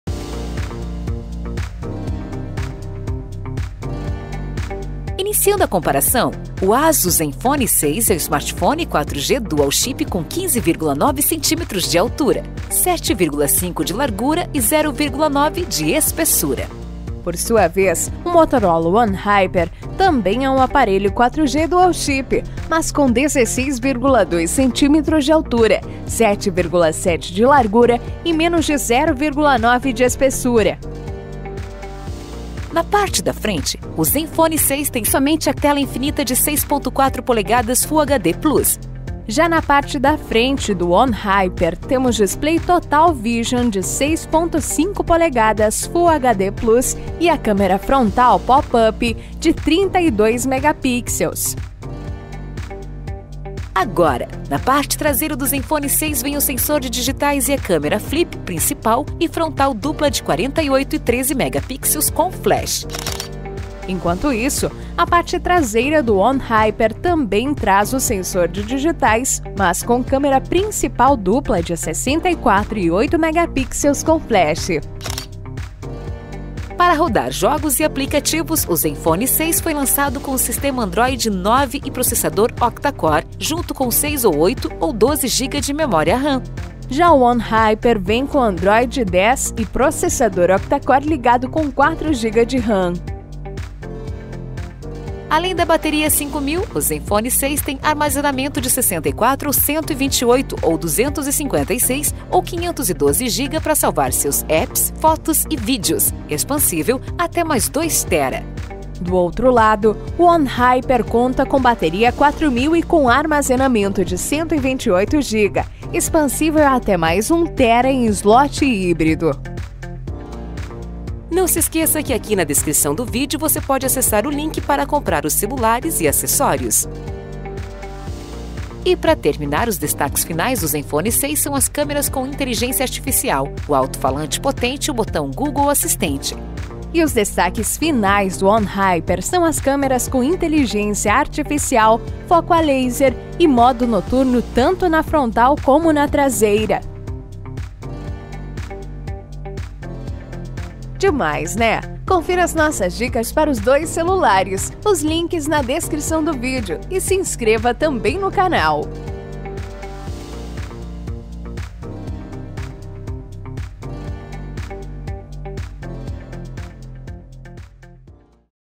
Zenfone 6 vs Motorola One Hyper (Comparativo)
Confira a ficha técnica do ASUS Zenfone 6 e do Motorola One Hyper, uma comparação de tamanho exclusiva (3D), e muito mais.

Links para comprar:

(Zenfone 6)